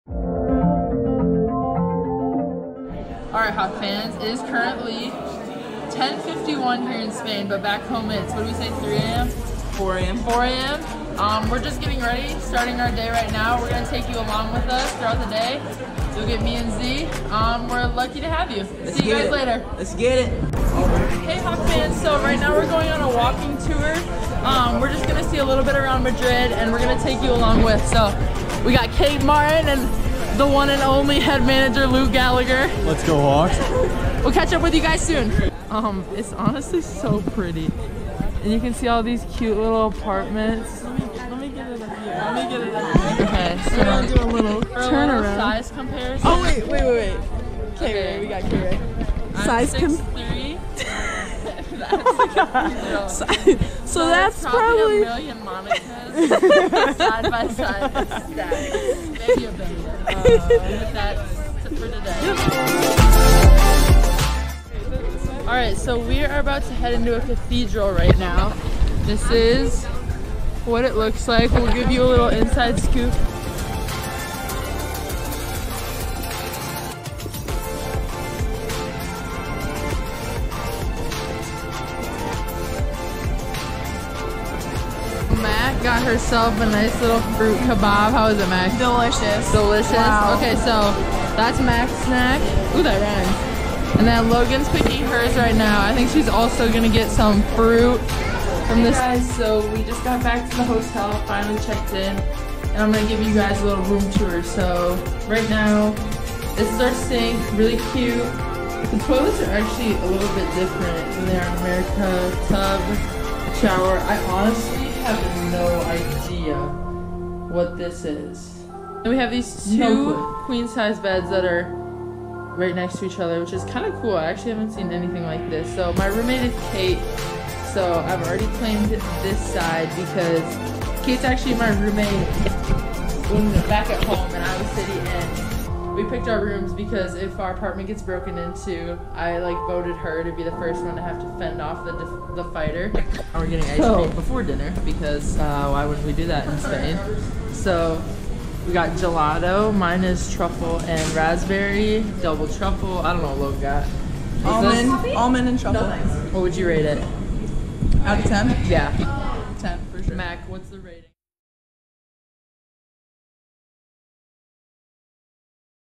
Spain Vlog: Day 1
Monika Czinano vlogs the team's first day in Spain